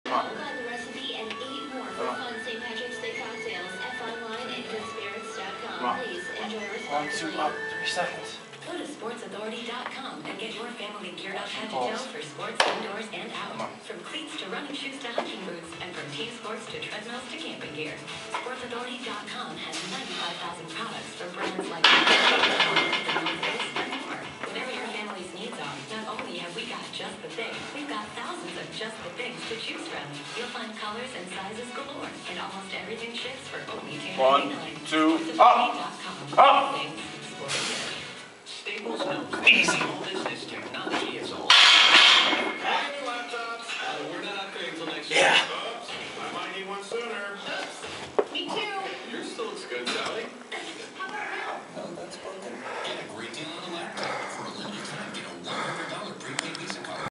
3 second pause squat - 530lbs. for 1 rep
2 - 100lb. plates, 1 - 35lb. plate on each side + 60lb. power bar

Just under 3x bodyweight for 1 - 3 second pause rep..performed  raw..no gear/equipment